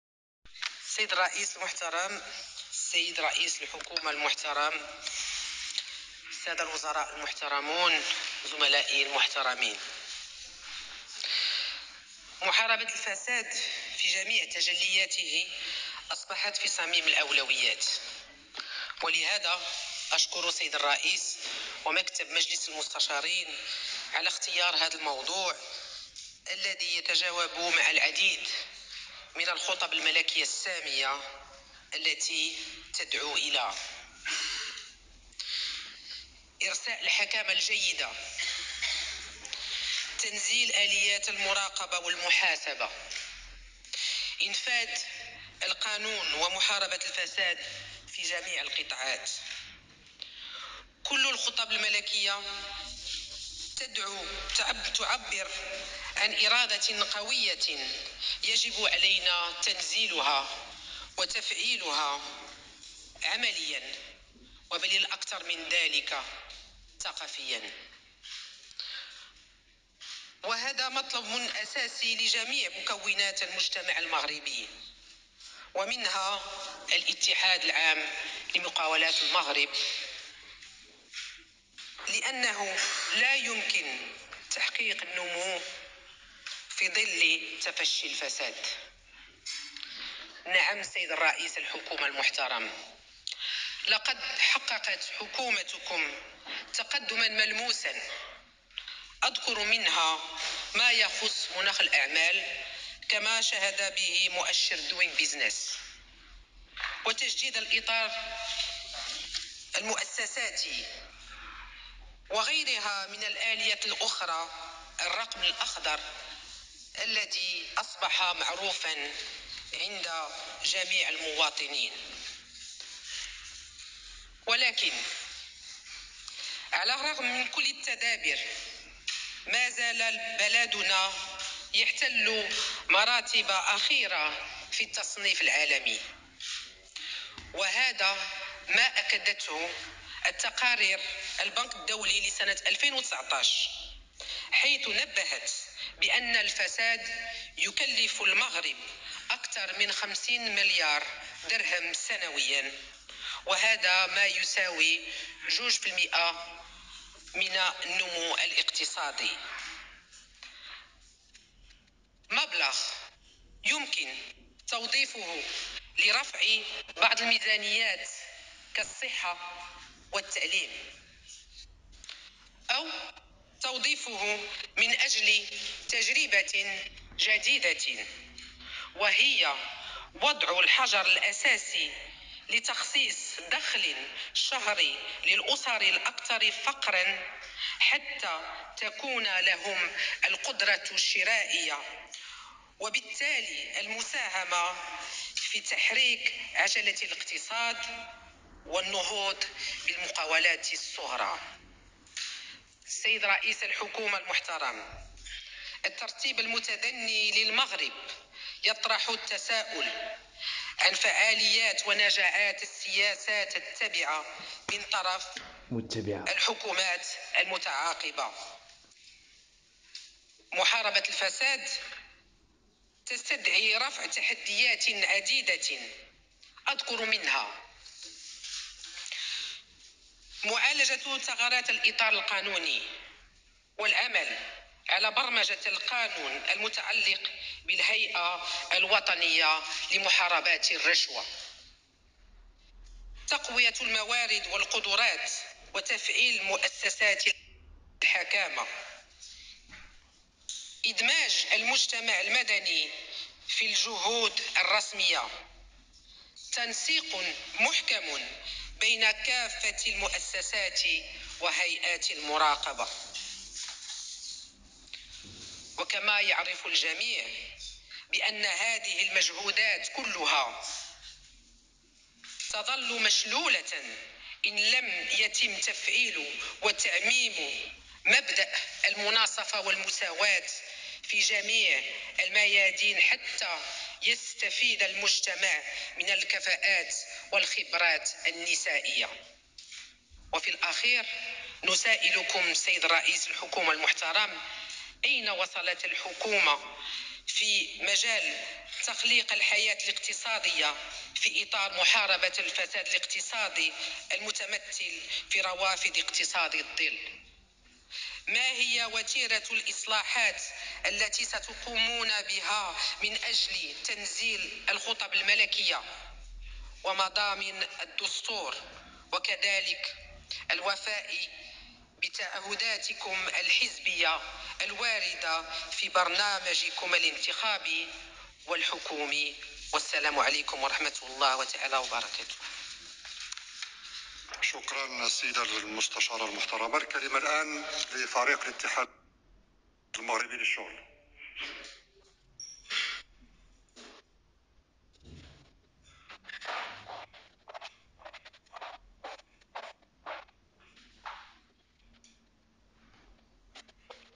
Neila Tazi plaide pour une politique anti corruption efficace
À la Chambre des conseillers , Neila TAZI interpelle le gouvernement sur le thème de la corruption